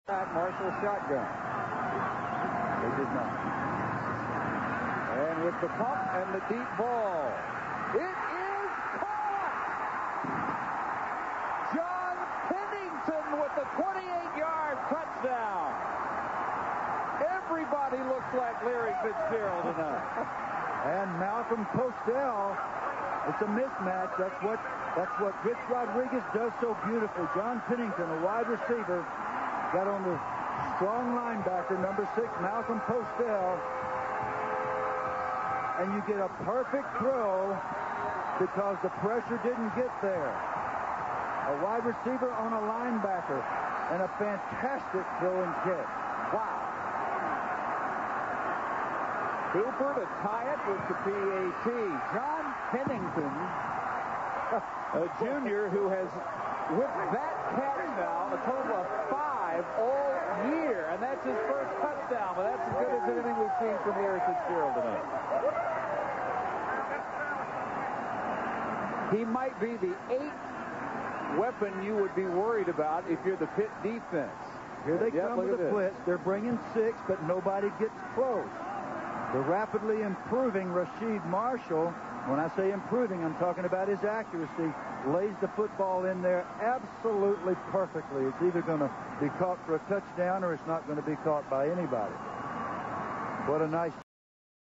2003 WVU vs Pitt: Rasheed Marshall to John Pennington
Rasheed Marshall throws a touchdown pass to John Pennington in WVU's 52-31 victory over Pitt on November 15, 2003.